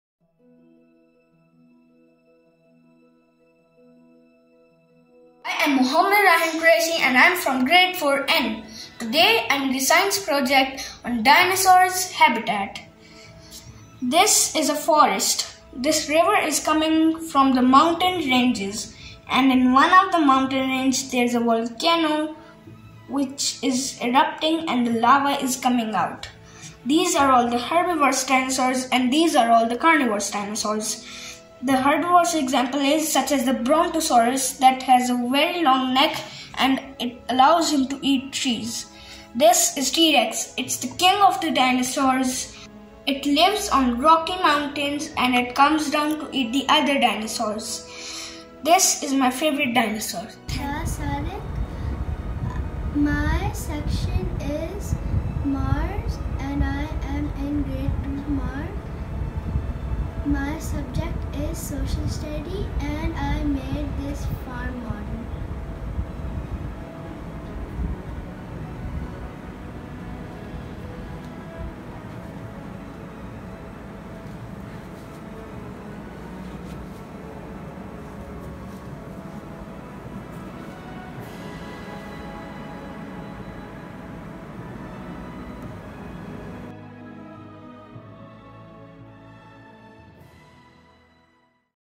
Hands on, Minds on Learning
A glimpse of Benchmark Junior School’s latest teaching paradigm “Hands on, Minds on Learning”